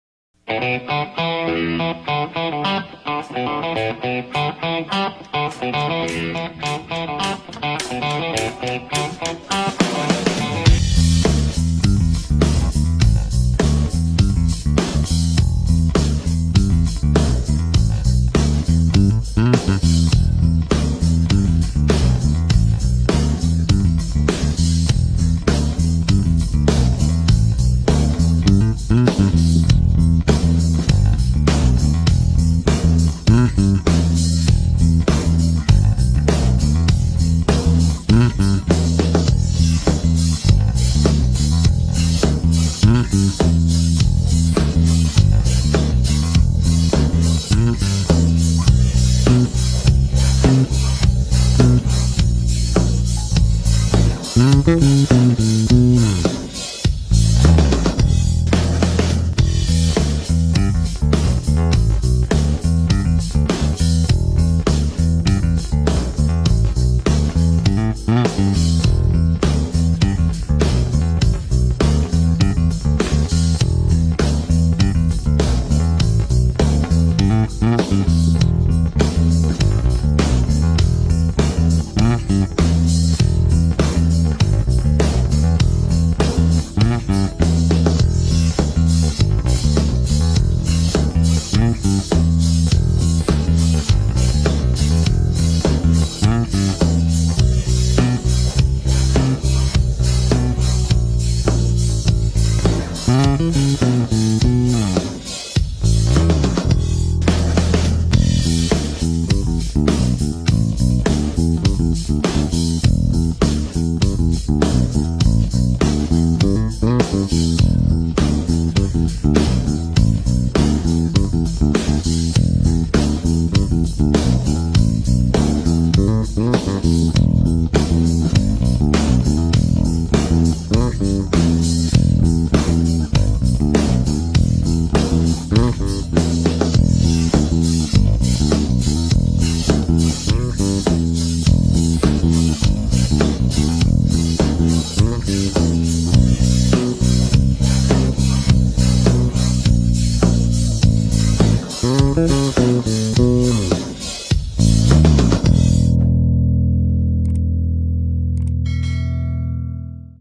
3 iconic bass sound comparison (no talking)
Fender Jazz American Pro2 vs Fender Precision American Pro2 vs Music Man Stringray Special  Sound Comparison